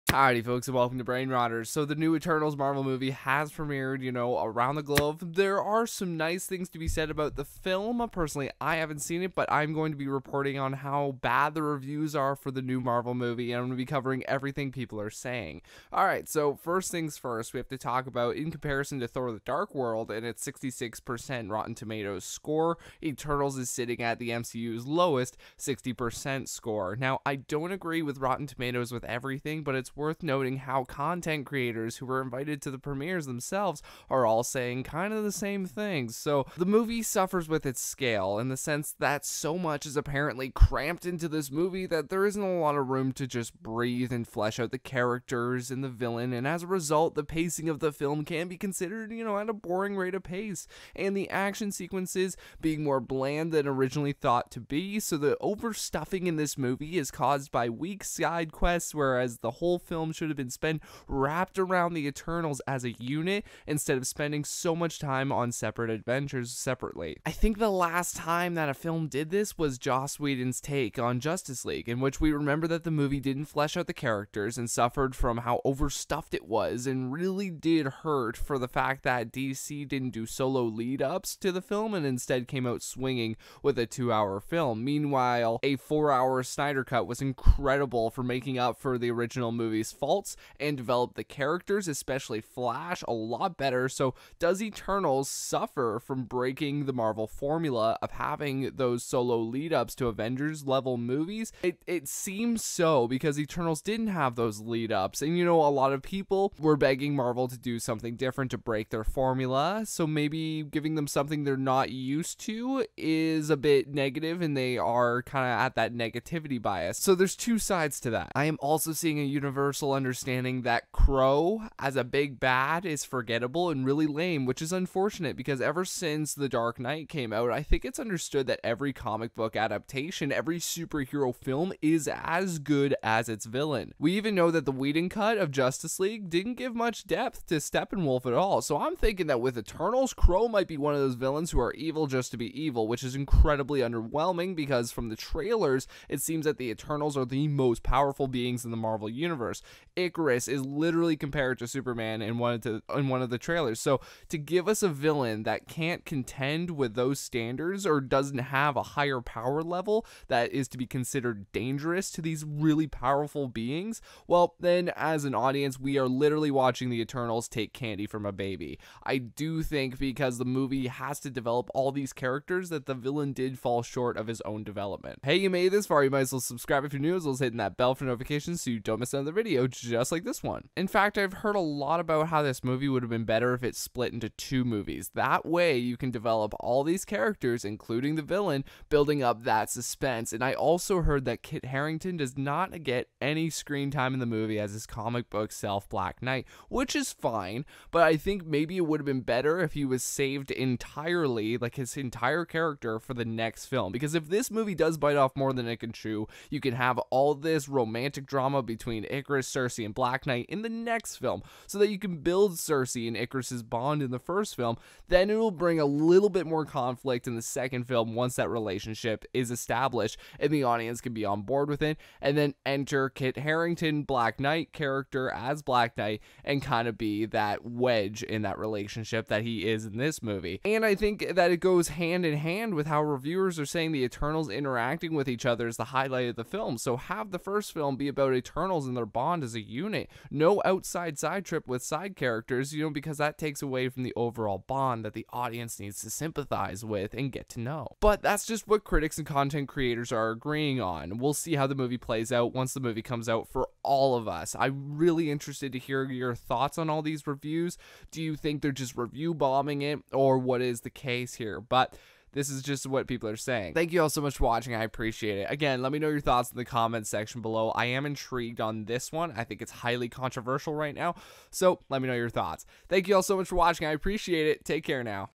The New Eternals Marvel Movie SUCKS?! + BAD Reviews Similar To Justice League 2017
So, the new Eternals movie has been featured at it’s red carpet premieres across the globe and there’s a pattern with how bad the reviews are for the new marvel movie so I’m going to be covering everything people are saying

Okay, so first things first, we have to talk about in comparison to Thor the Dark World and its 66% rotten tomatoes score, Eternals is sitting at the MCU’s lowest 60% score - now I don’t agree with Rotten Tomatoes with everything but it’s worth noting how content creators who were invited to the premieres are all saying the same things:
Such as, first: The movie suffers with its scale, in the sense that so much is apparently cramped into this movie that there isn’t a lot of room to breathe and flesh out the characters as a result the pacing of the film can be considered at a boring rate of pace and the action sequences being more bland than originally thought to be - so the overstuffing in this movie is caused by weak side quests whereas the whole film should’ve been spent wrapped around the eternals as a unit instead of spending so much time on separate adventures - I think the last time that a film did this was Joss Whedon’s take on Justice League in which we remember that the movie didn’t flesh out the characters, and suffered from how overstuffed it was and really did hurt for the fact that DC didn’t do solo lead-ups to the film and instead came out swinging with a two hour film - meanwhile a four hour Snyder Cut was incredible for making up for the original movie’s faults and developed the characters, especially Flash a lot better - so does Eternals suffer from breaking the marvel formula of having those solo lead ups to Avengers level movies? It seems so.
I am also seeing a universal understanding that Kro as a big bad is forgettable, and really lame which is unfortunate because ever since The Dark Knight came out, I think it’s understood that every comic book adaptation, every superhero film is as good as its villain. We even know that the Whedon Cut of Justice League didn’t give much depth to Steppenwolf at all - so I’m thinking that with Eternals, Kro might be one of those villains who are evil just to be evil which is extremely underwhelming because form the trailers it seems that the eternals are the most powerful beings in the marvel universe, Ikaris is literally compared to superman in one of the trailers so to give us a villain that can’t contend with those standards or doesn’t have a higher power-level, well than we as an audience are literally watching the eternals take candy from a baby. I do think because the movie has to develop all of these characters that the villain did fall short of his own development
In fact, I’ve heard a lot about how this movie would’ve been better if split into two movies that way you can develop all these characters, including the villain, building up that suspense, and I also heard that Kit Harington does not get any time in the movie as his comic book self Black Knight, which is fine, but I think maybe it would’ve been better if he was saved for the next film entirely because if this movie does bite off more than it can chew, you can have all this romantic drama between Ikaris, Sersi, and Black Knight in the next film, so that you can build Sersi and Ikaris’ bond in the first film, than it will bring a little more conflict in the second film once that relationship is established and the audience can be onboard with it. And I think that goes hand in hand with how reviewers are saying the eternals interacting with each other is the highlight of the film, so have that first film be about the eternals and their bond as a unit - no outside side trips with side characters because that takes away form the overall bond that the audience needs to sympathize with.

But, that’s just what critics and content creators are agreeing on, we’ll see how the movie plays out once the movie comes out.